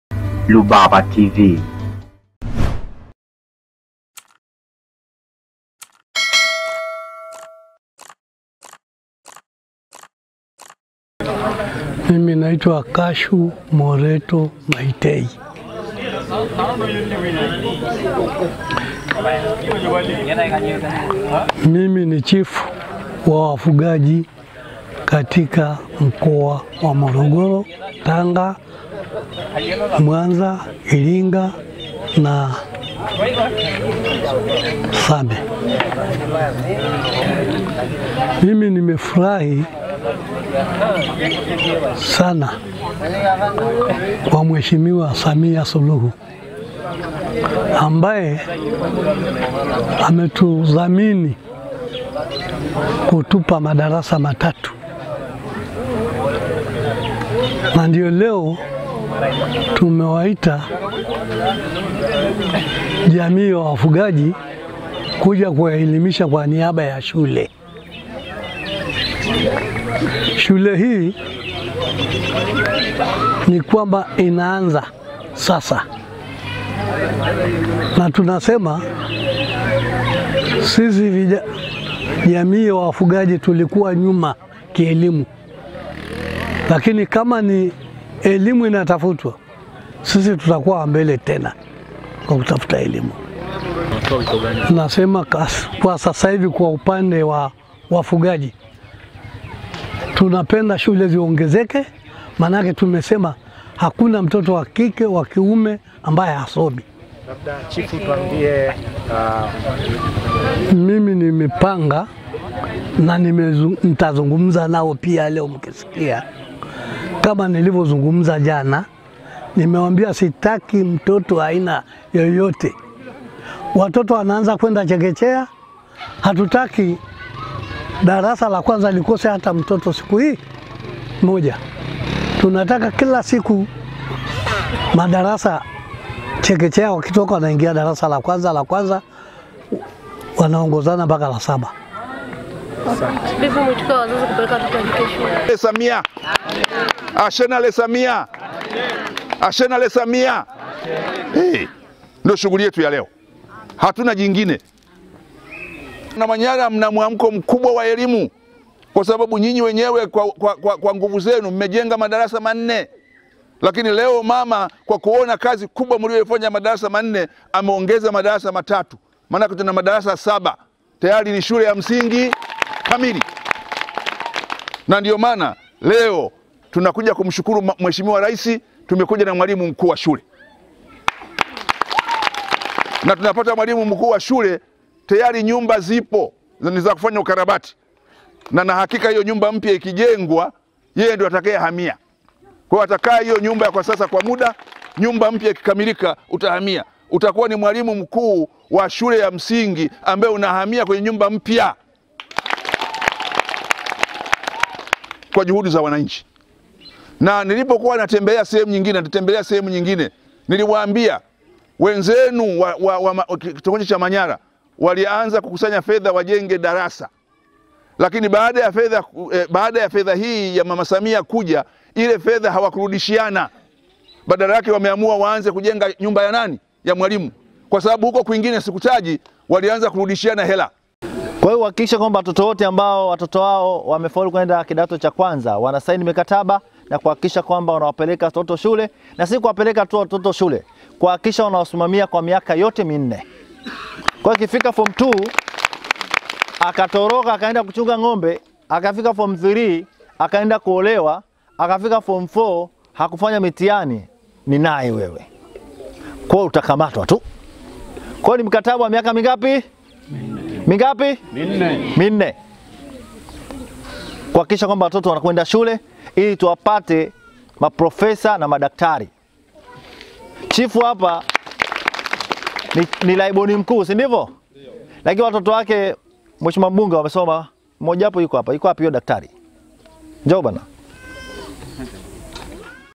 WAFUGAJI WAMPONGEZA RAIS SAMIA KWA KUWAKUMBUKA UJENZI MADARASA
Wafugaji jamii  ya Kimasai Kitongoji cha Manyara kijiji cha Twatwatwa  wilaya ya Kilosa Mkoani Morogoro wamempongeza Rais Samia kwa kuwakumbuka jamii hiyo ambayo ilisahaulika katika sekta ya elimu kwa kuwajengea madarasa matatu kupitia  mradi wa Covid 19

Akizungumza katika hafla hiyo Chifu mkuu wa  mikoa saba wa Kabila hilo la Wamasai  Chifu Kashu Moreto amesema wanamshukuru Rais Samia kwa  na kuahidi kumuunga mkono

Kwa upande wake mkuu wa wilaya ya Kilosa Alhj Majd Mwanga amewataka wazazi kuacha tabia ya kuwaozesha watoto wa kike  huku Mbunge wa Kilosa Profesa Palamagamba Kabudi akiahidi kushirikiana na jamii hiyo katika kuinua elimu kwa jamii hiyo ya kifugaji.